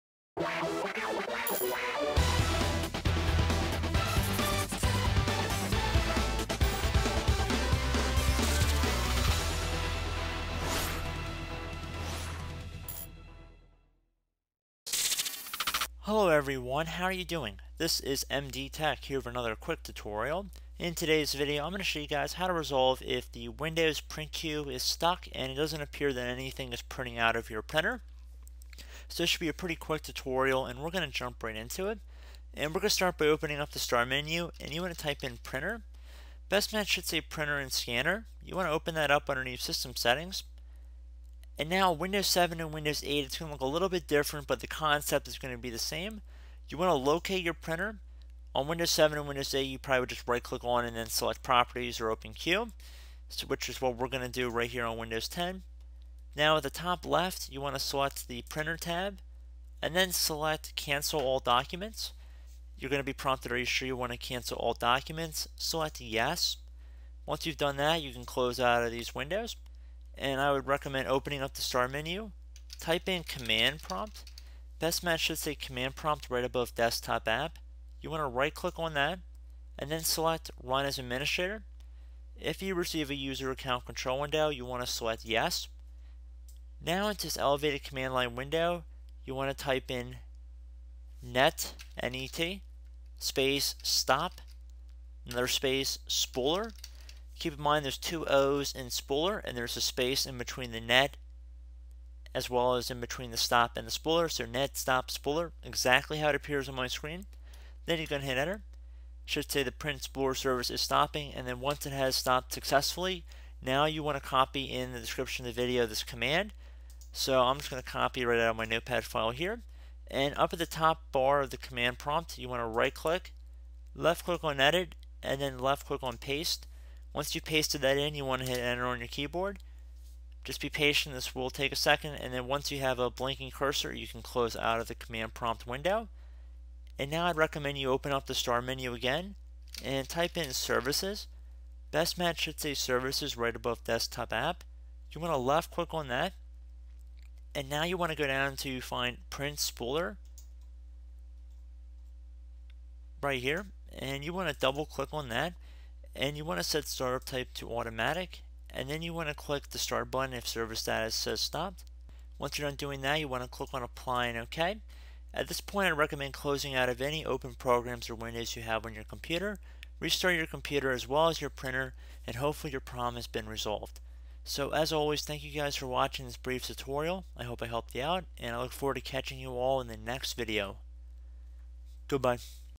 Reset Printer When Your Print Job Is Stuck in the Windows Print Queue
How to restart the Print Spooler on Windows 10

Commands used in this tutorial:

"Net Stop Spooler."

Type this command:
del %systemroot%\System32\spool\printers\* /Q

Printers can be very finicky devices, especially when it comes to the print queue. Have you ever set a few items to print only to check the queue and see that a failed or stuck print job is holding up the entire process? Not only that, you can't delete or cancel the stuck print job from the queue, which is seriously frustrating.

Sometimes, documents you’re printing get stuck in the printer’s queue, preventing further documents from being printed. Here’s how to fix it when that happens.

Whether you’re using a local or shared network printer, sometimes printing doesn’t go quite right. If you’ve tried troubleshooting obvious printer problems—paper jams, no paper, low ink or toner, or simply restarting the printer—it’s time to turn your attention toward the print queue. Often, simply clearing and restarting the print spooler—the software that prepares and manages printing documents—can fix the problem. If that fails, you may need to cancel one or more documents in your print queue and see if that gets things going again.

To get the queue unglued and your documents printing again, here's how to cancel a stuck print job in Windows 10 when conventional methods won't work.